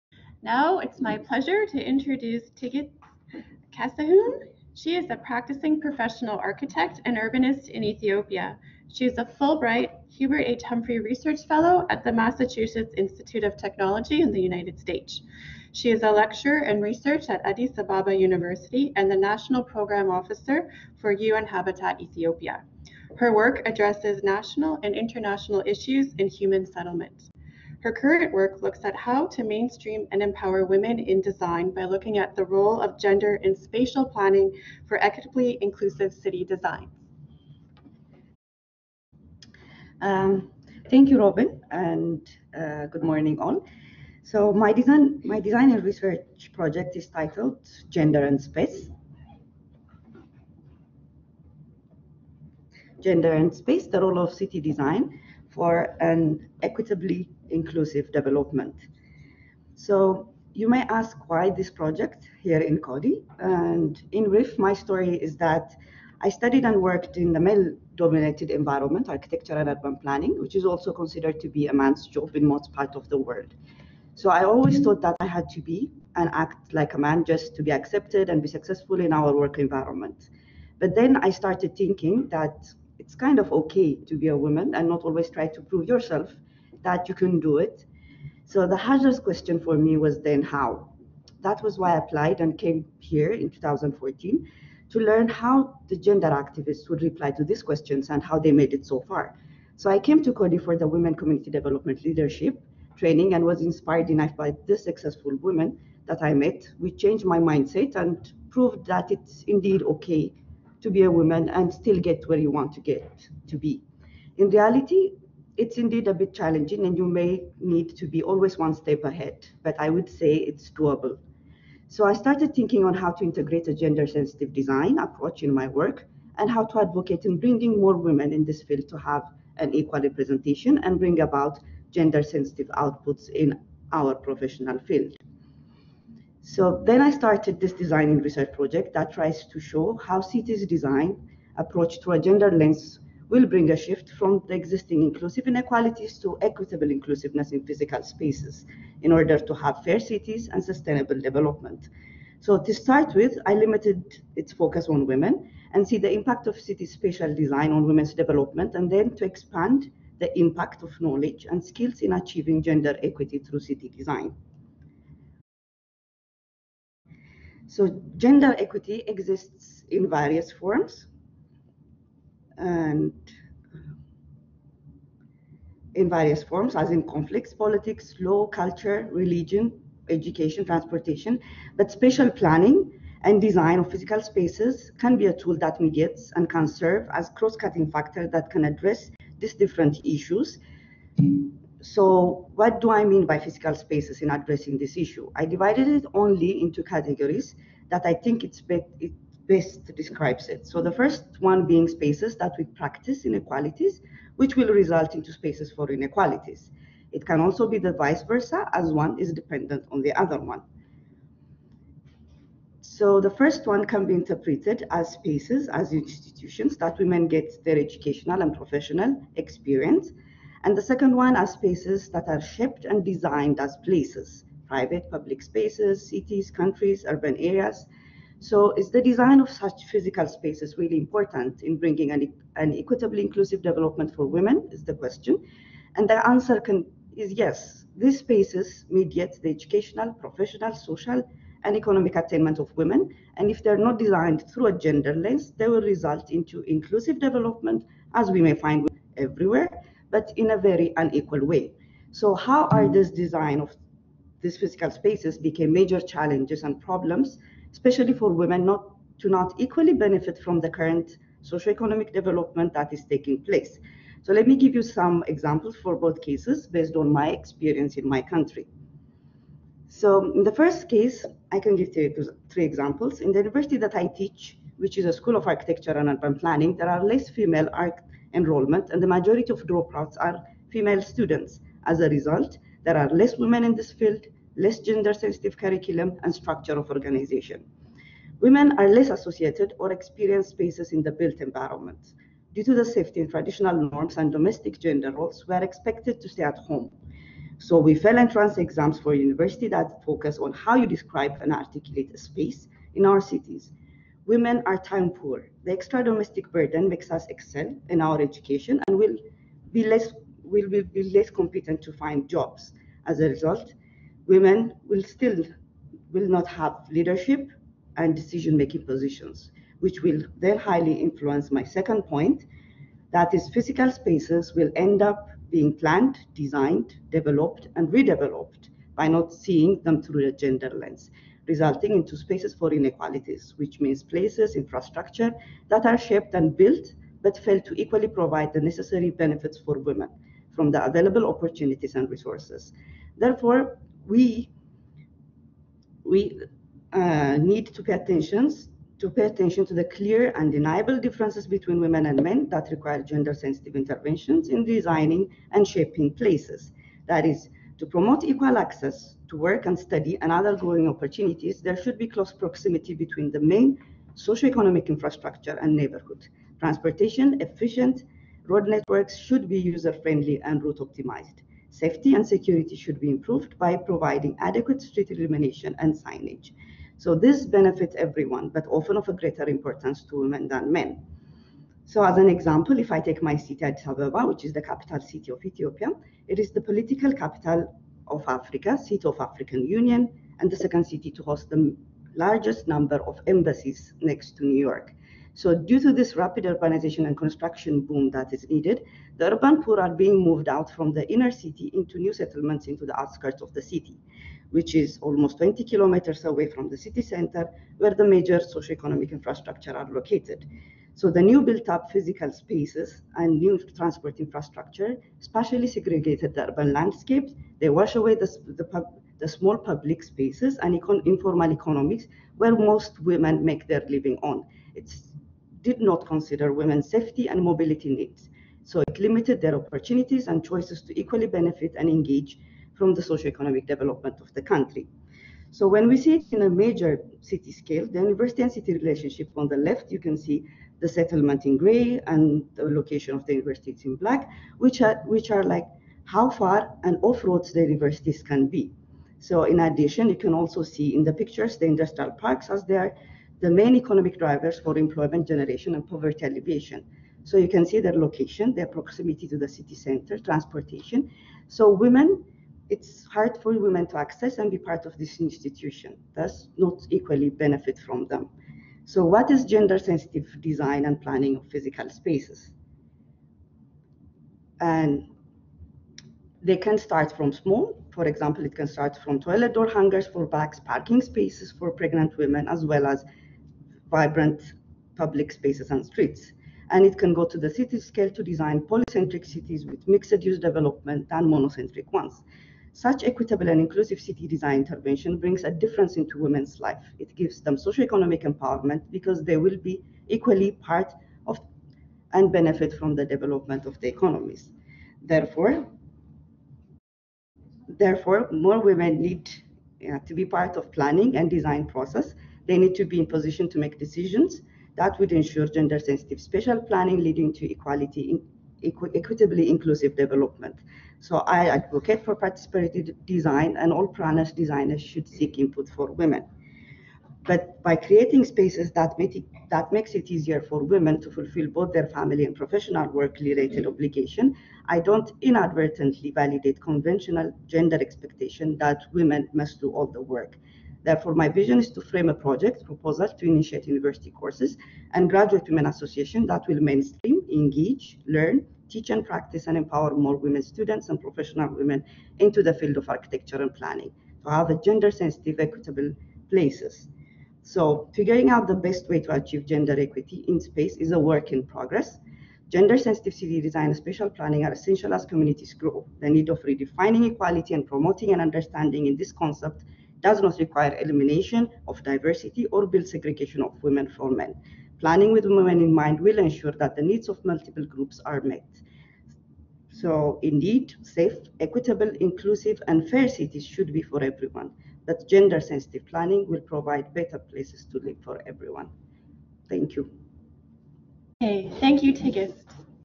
Coady Webinar - Gender and Space: ​ The role of city design for an equitably inclusive development
Gender and Space: ​The role of city design for an equitably inclusive development
A Coady Connects Webinar

Tigist Kassahun Temesgen​
COADY 2018 ​Women’s Leadership Research Fellow​